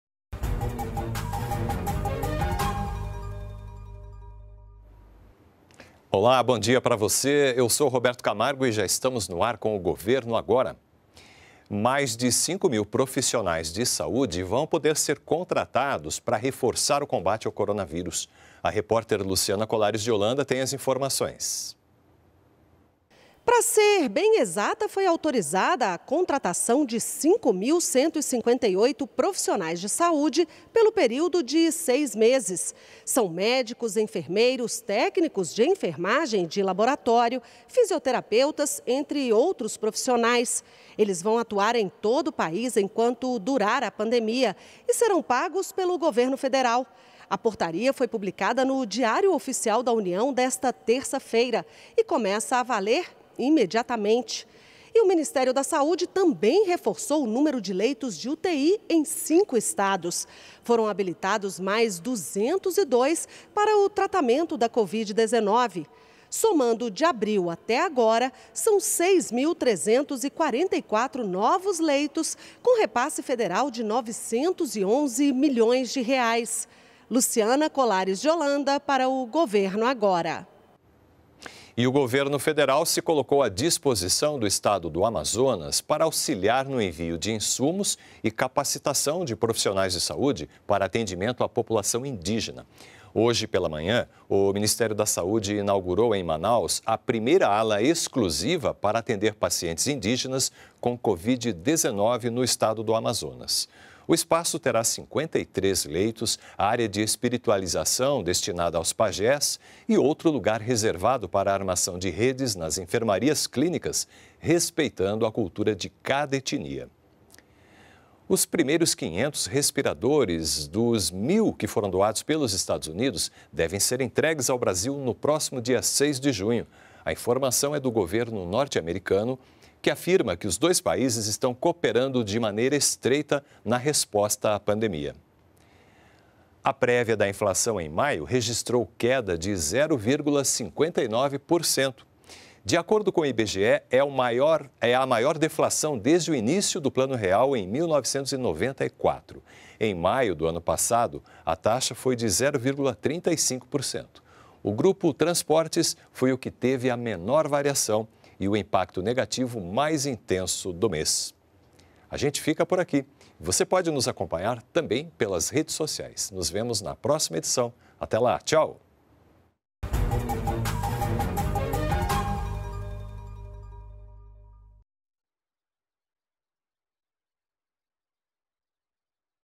500 respiradores devem ser entregues no próximo dia 06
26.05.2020 - Mais de 5 mil profissionais do setor da saúde vão poder ser contratados para reforçar o combate à Covid-19. E mais: os primeiros 500 respiradores dos 1000 doados pelos Estados Unidos ao Brasil devem ser
entregues no próximo dia 06 de junho.